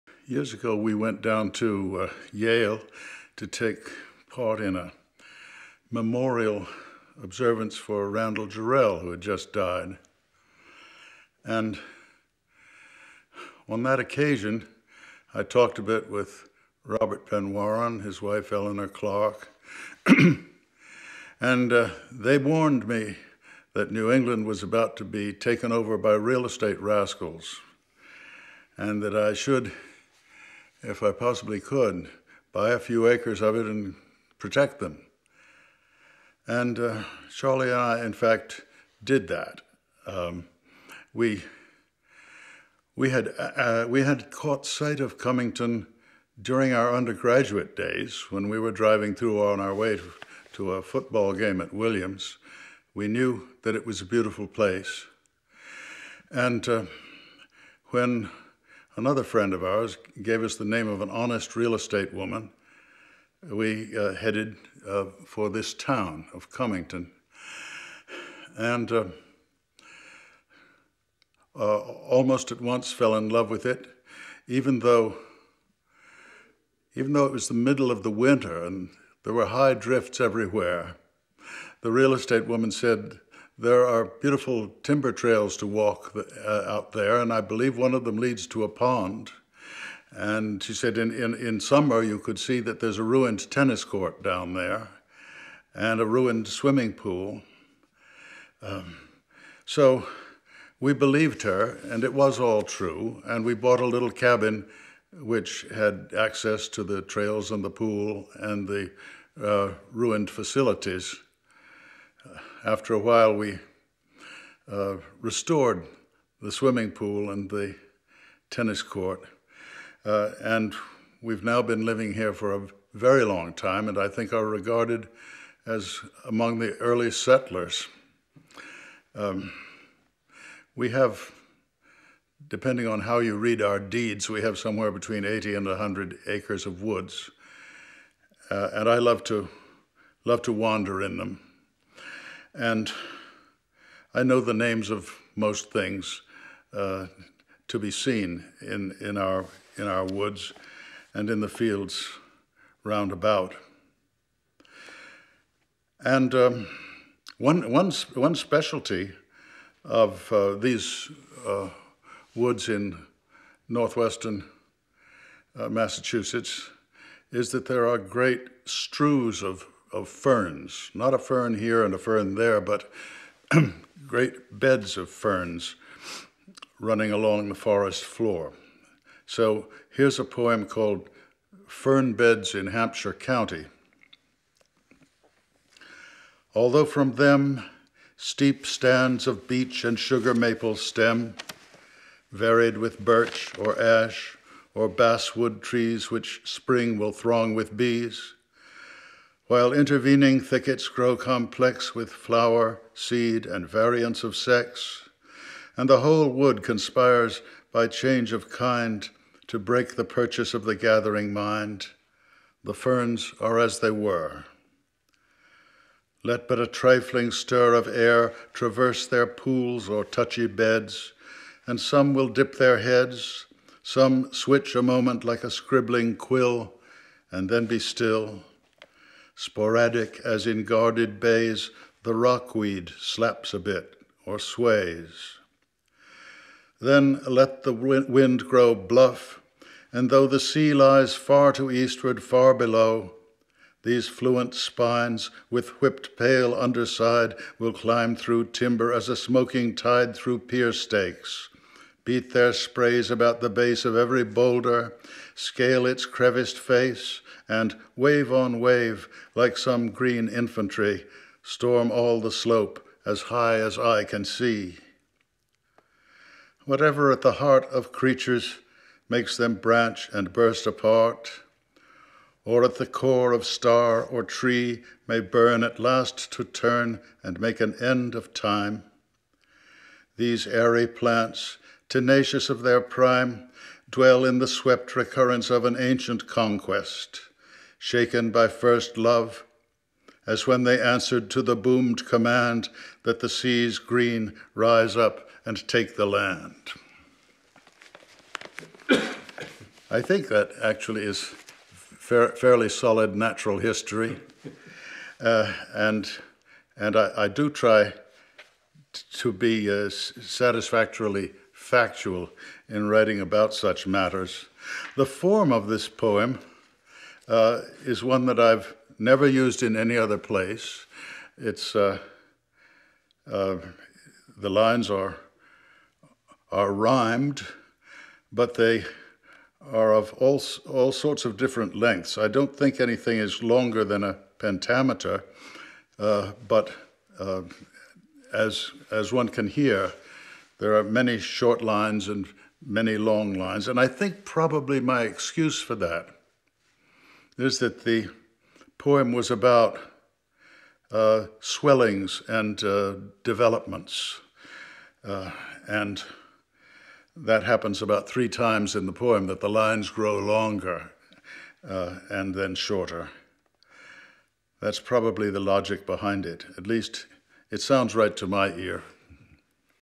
Richard Wilbur - Buying property in Cummington inspires 'Fern-Beds in Hampshire County' (69/83)
Acclaimed US poet Richard Wilbur (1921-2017) was the second US Poet Laureate and twice received the Pulitzer Prize for Poetry, in 1957 and 1989. Educated at Harvard University, he also served in the Army during World War II. [Listener: David Sofield; date recorded: 2005]

TRANSCRIPT: Years ago we went down to Yale to take part in a memorial observance for Randall Jarrell who had just died, and on that occasion I talked a bit with Robert Penn Warren, his wife, Eleanor Clark and they warned me that New England was about to be taken over by real estate rascals and that I should, if I possibly could, buy a few acres of it and protect them, and Charlee and I in fact did that. We... we had, we had caught sight of Cummington during our undergraduate days when we were driving through on our way to a football game at Williams. We knew that it was a beautiful place and when another friend of ours gave us the name of an honest real estate woman, we headed for this town of Cummington, and almost at once fell in love with it, even though... even though it was the middle of the winter and there were high drifts everywhere, the real estate woman said, 'There are beautiful timber trails to walk out there and I believe one of them leads to a pond', and she said, 'In summer you could see that there's a ruined tennis court down there and a ruined swimming pool'. So, we believed her and it was all true and we bought a little cabin, which had access to the trails and the pool and the ruined facilities. After a while we restored the swimming pool and the tennis court, and we've now been living here for a very long time and I think are regarded as among the early settlers. We have - depending on how you read our deeds - we have somewhere between 80 and 100 acres of woods, and I love to... love to wander in them, and I know the names of most things to be seen in our... in our woods and in the fields roundabout, and one... one specialty of these woods in North Western Massachusetts is that there are great strews of... of ferns, not a fern here and a fern there, but great beds of ferns running along the forest floor. So, here's a poem called Fern-Beds in Hampshire County.

Although from them
Steep stands of beech and sugar-maple stem,
Varied with birch, or ash, or basswood trees
Which spring will throng with bees,
While intervening thickets grow complex
With flower, seed, and variants of sex,
And the whole wood conspires, by change of kind,
To break the purchase of the gathering mind,
The ferns are as they were.
Let but a trifling stir
Of air traverse their pools or touchy beds
And some will dip their heads,
Some switch a moment like a scribbling quill
And then be still,
Sporadic as in guarded bays
The rockweed slaps a bit or sways.
Then, let the wind grow bluff, and though
The sea lies far to eastward, far below,
These fluent spines, with whipped pale underside,
Will climb through timber as a smoking tide
Through pier-stakes, beat their sprays about the base
Of every boulder, scale its creviced face
And, wave on wave, like some green infantry,
Storm all the slope as high as I can see.
Whatever at the heart
Of creatures makes them branch and burst apart,
Or at the core of star or tree may burn
At last to turn
And make an end of time,
These airy plants, tenacious of their prime,
Dwell in the swept recurrence of
An ancient conquest, shaken by first love
As when they answered to the boomed command
That the seas green rise up and take the land.
I think that actually is fairly solid natural history, and... and I do try to be satisfactorily factual in writing about such matters. The form of this poem is one that I have never used in any other place. It's... the lines are rhymed, but they are of all sorts of different lengths. I don't think anything is longer than a pentameter, but as... as one can hear, there are many short lines and many long lines, and I think probably my excuse for that is that the poem was about swellings and developments, and that happens about three times in the poem that the lines grow longer and then shorter. That's probably the logic behind it, at least, it sounds right to my ear.